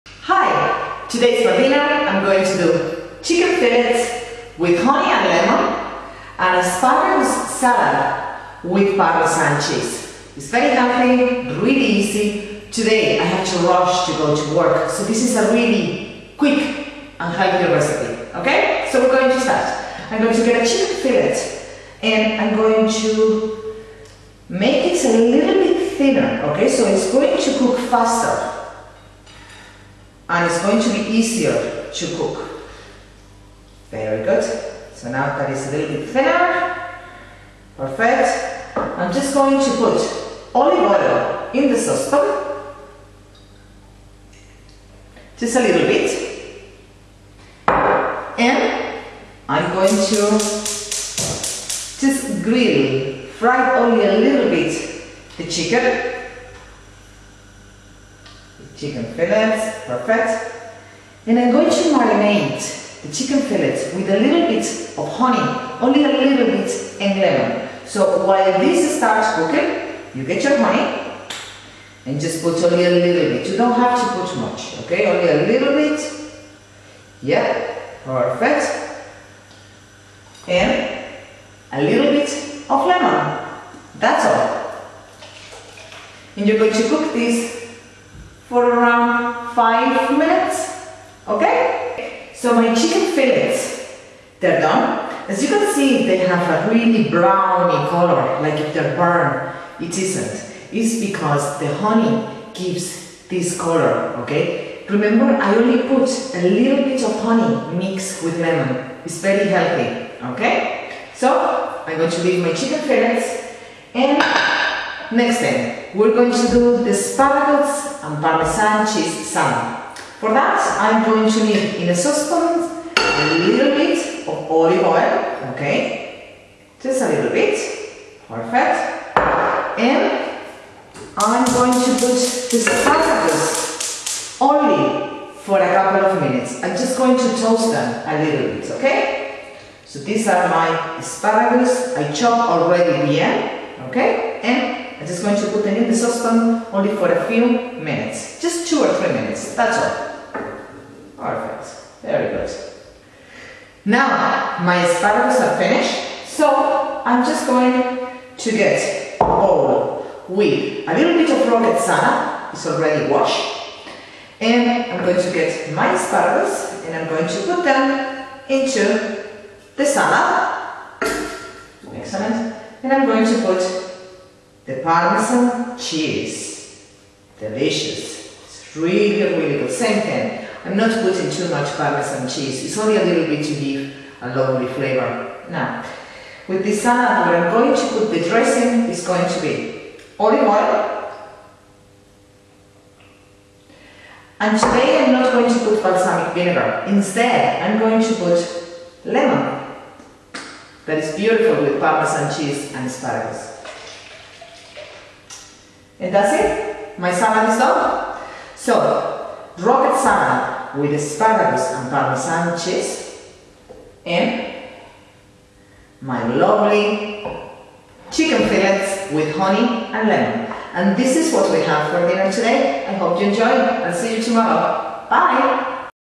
Chicken with honey & lemon and asparagus with parmesan cheese salad.
is quick , healthy and easy recipe
Ingredients , chicken fillets, honey, lemon , for the salad, rocket salad, parmesan cheese. asparagus,and olive oil, black peper